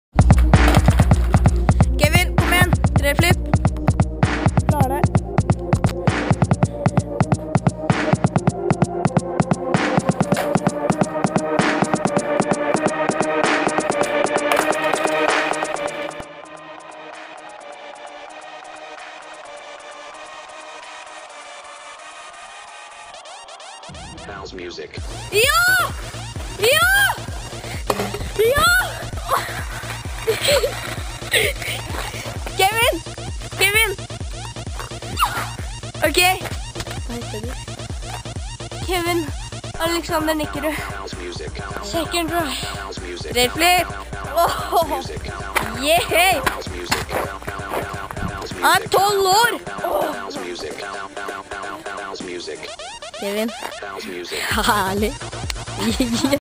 Kevin 360flip Mjøndalen
Dette spottet er ved Jula mjøndalen jeg digger det!